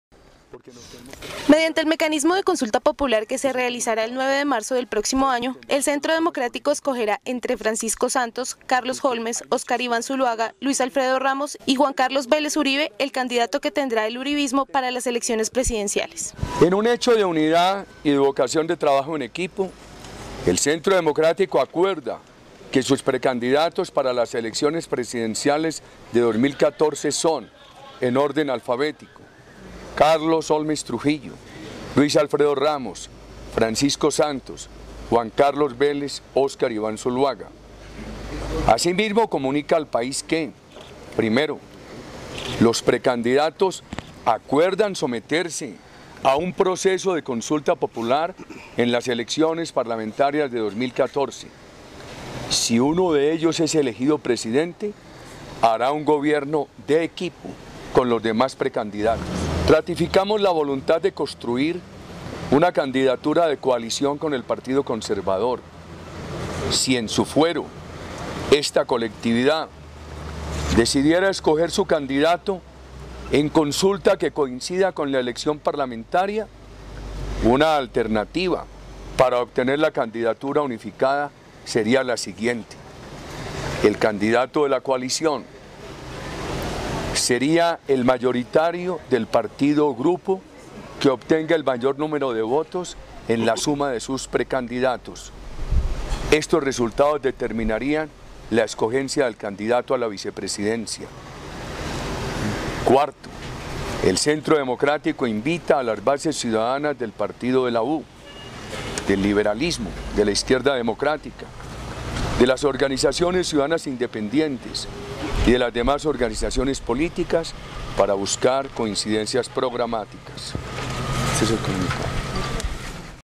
Por consulta popular se eligirá el candidato presidencial uribista
Entre Francisco Santos, Óscar Iván Zuluaga, Carlos Holmes Trujillo, Luis Alfredo Ramos y Juan Carlos Vélez, se escogerá el aspirante presidencial del Centro Democrático. Uribe llama al conservatismo a coalición.